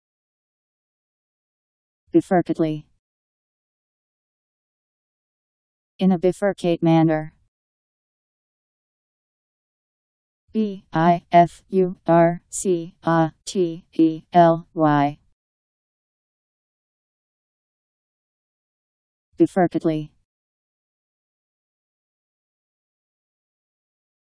Bifurcately Meaning
Video shows what bifurcately means. In a bifurcate manner..  Bifurcately Meaning. How to pronounce, definition audio dictionary. How to say bifurcately. Powered by MaryTTS, Wiktionary